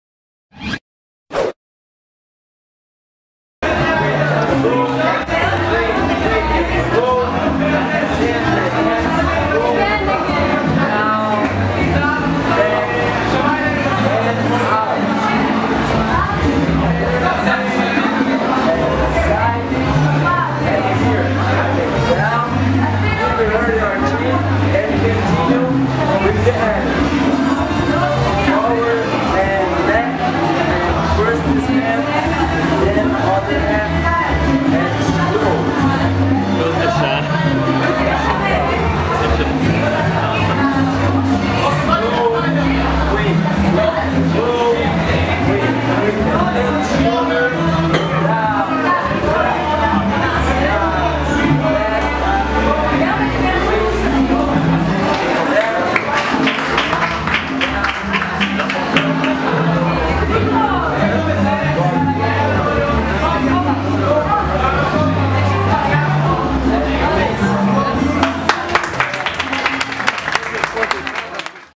Brazilian Zouk workshop with Michael & Aline
Brazilian Zouk workshop with Michael Boy & Aline Borges at The Complex, Tel-Aviv, Israel.